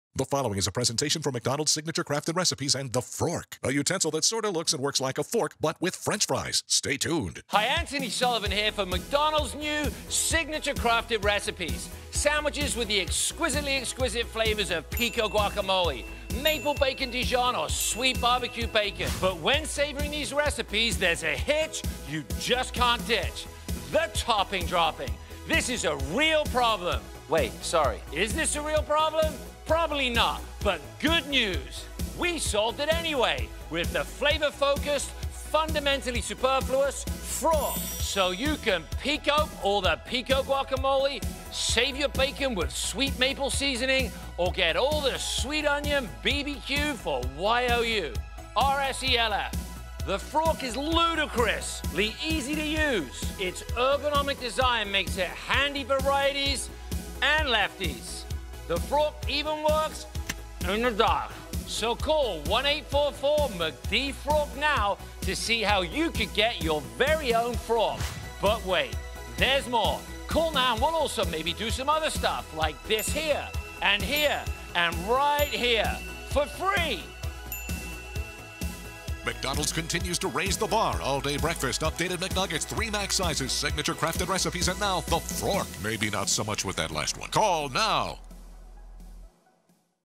Introducing The Frork™: McDonald's Latest Invention ft. Anthony Sullivan
McDONALD’S AND ANTHONY SULLIVAN HERE (yes, THAT Anthony Sullivan) for our new favorite utensil. It’s like a fork, but with fries… a fry fork… a Frork™! It’s the perfect(ish) utensil for picking up any premium toppings that may fall out of our new Signature Crafted Recipes burgers and chicken sandwiches.

And that’s not all! to find out how you might get your very own Frork™.

We can’t WAIT to show you what’s next!